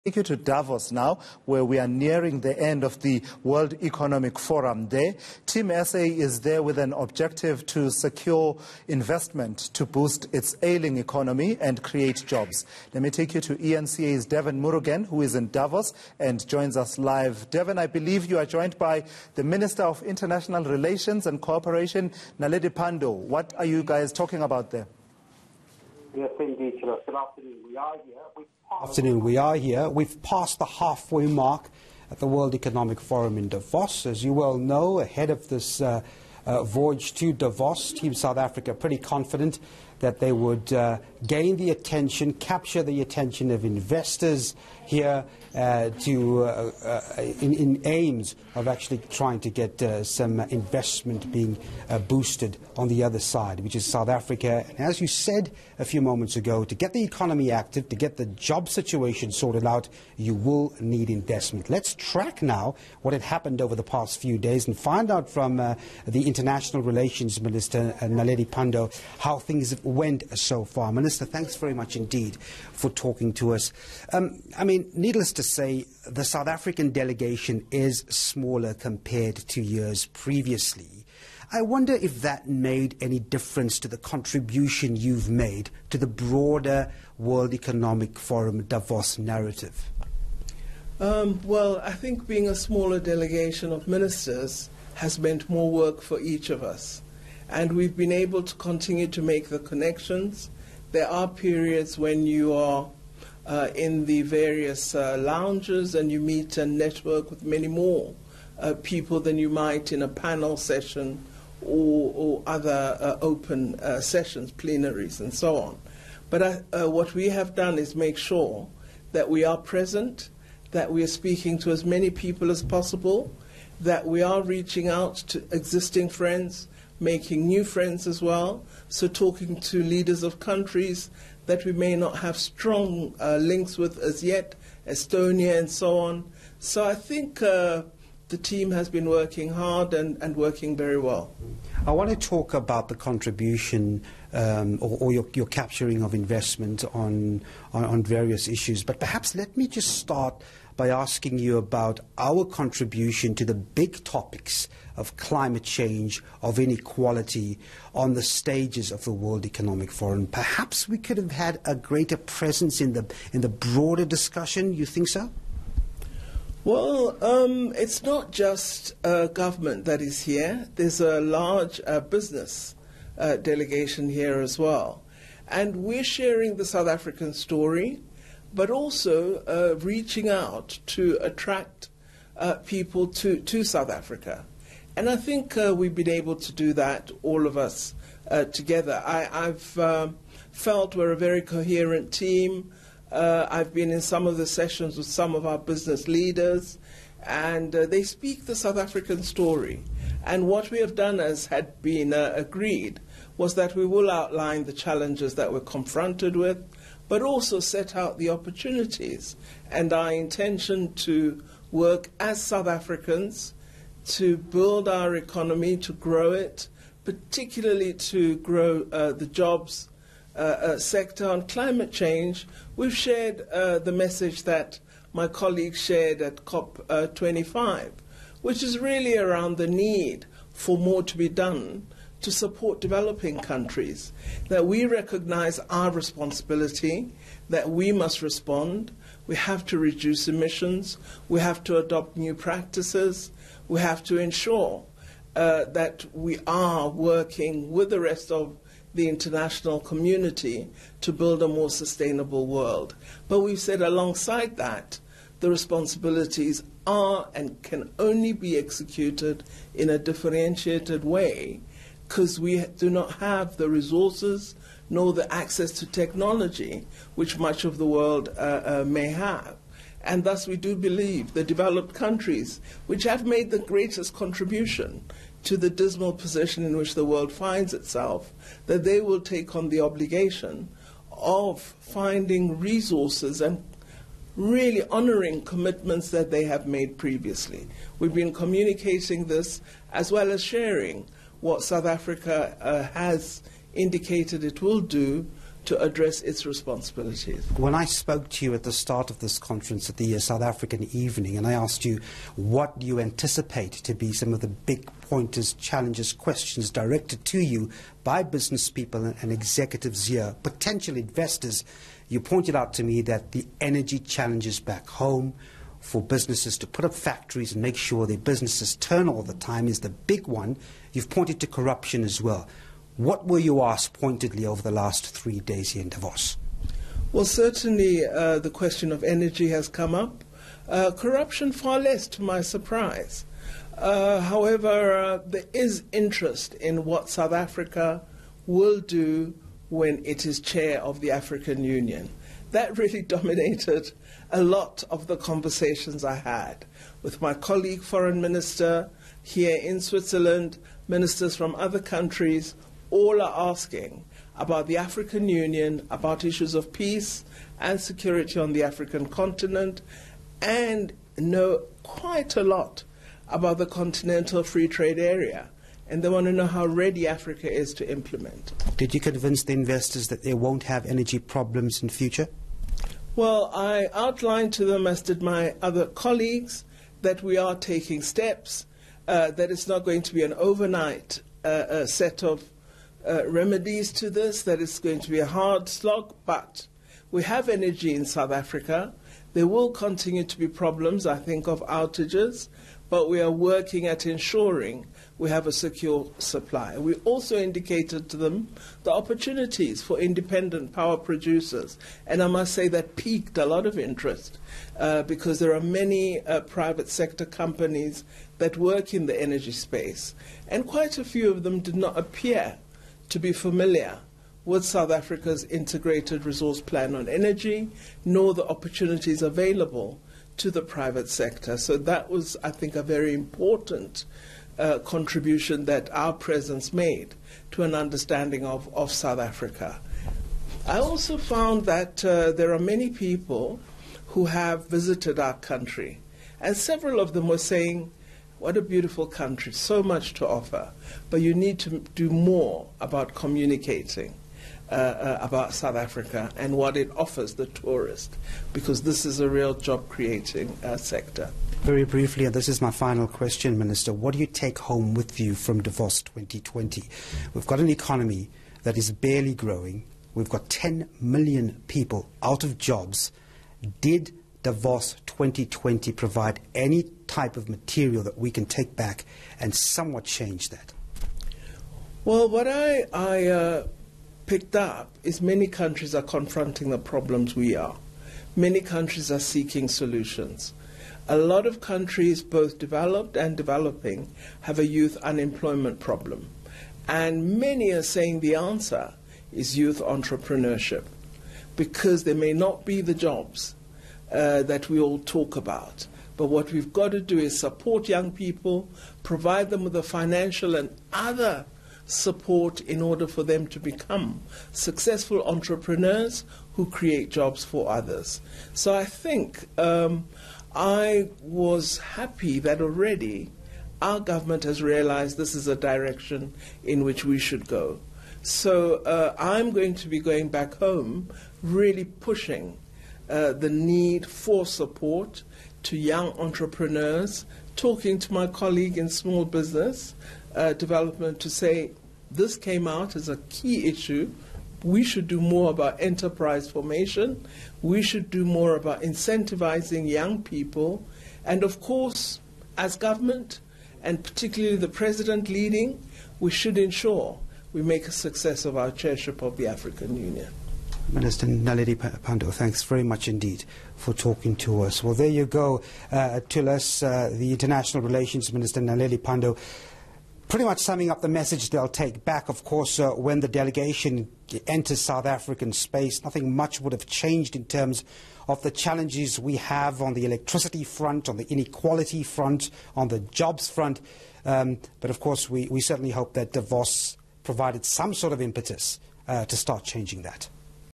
In Conversation with Naledi Pandor at the World Economic Forum
Team SA is there with an objective to securing investment to boost its ailing economy and create jobs. Courtesy